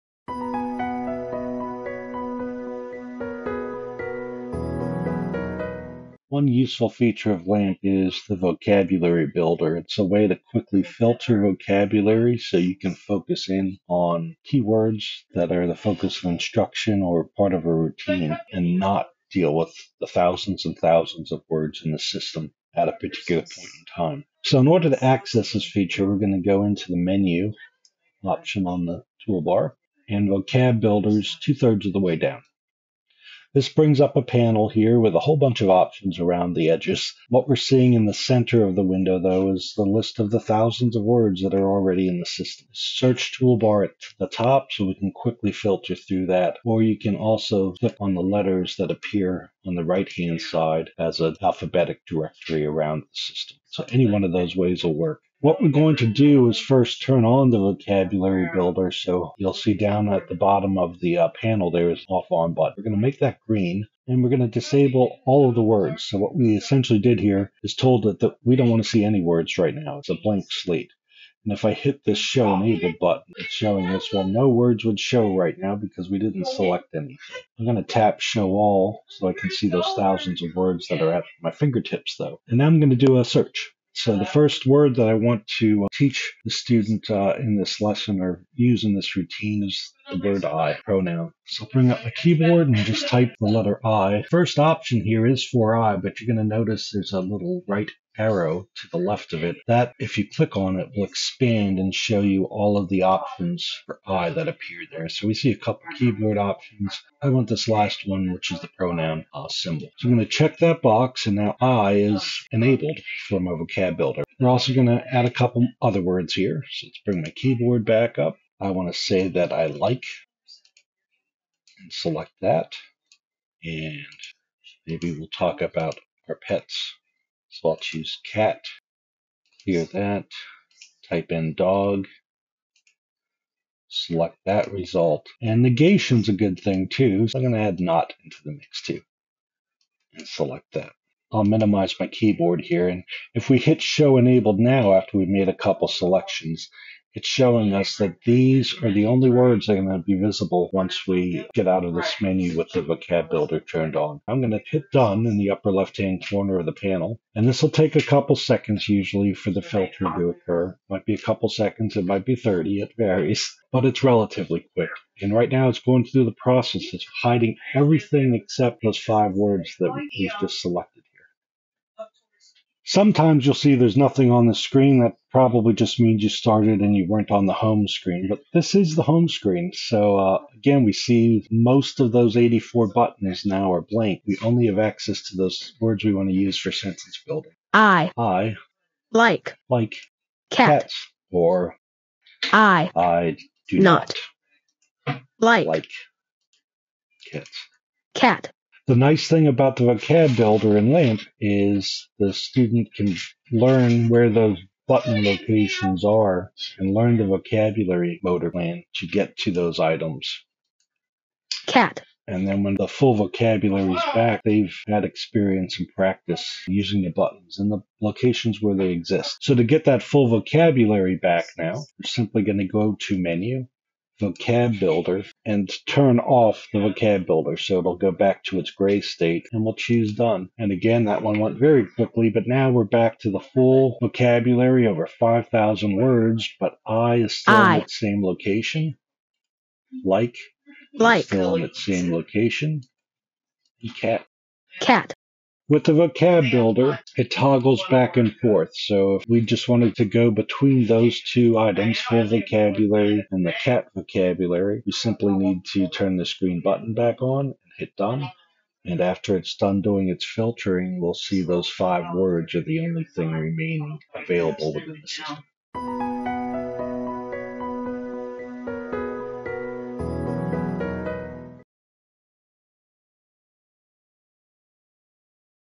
SQT 025 Using Vocabulary Builder in LAMP
This quick video will guide viewers through the use of the Vocabulary Builder feature of LAMP Words for Life to efficiently and temporarily hide and reveal vocabulary when instructing and implementing with LAMP.